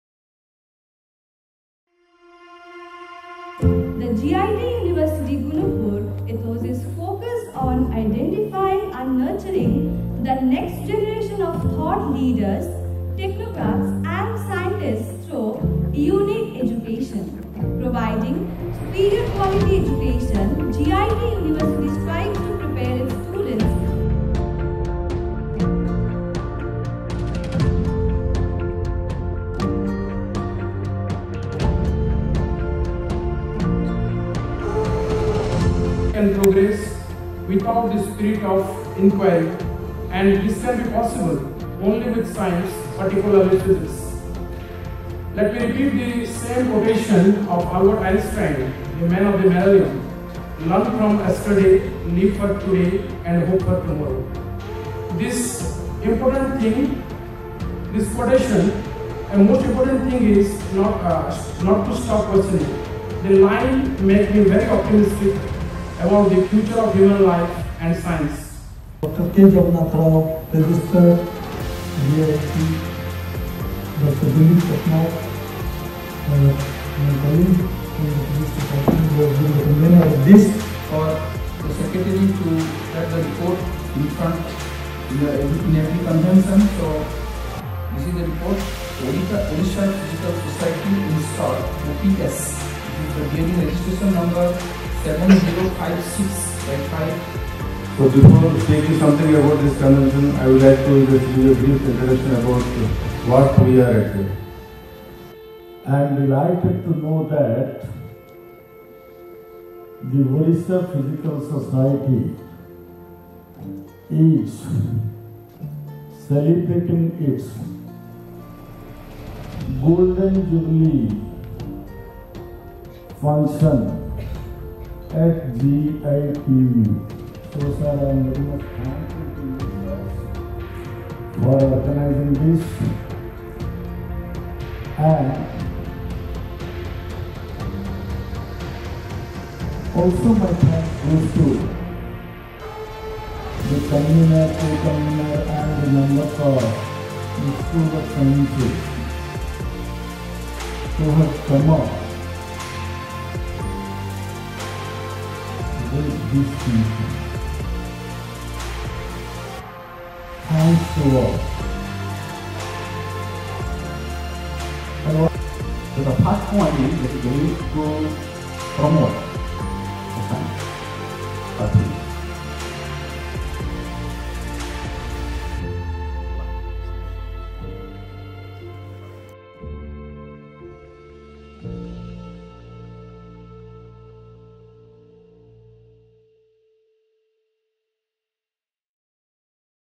Golden Jubilee Celebration of OPS - National Seminar on Physics in Sustainable Development
Department of Basic science, Humanities & School of Sciences, GIET University, Gunupur has organised the 38th convention of Orissa Physical Society (Golden Jubilee celebration of OPS) along with the “National Seminar on Physics in Sustainable Development” during 4th-5th Jun, 2022.

The Seminar covered all the areas of Physics with an aim to provide the platform for Academicians, Scientists and Researchers to share the latest development and its applications to technology for sustainable development.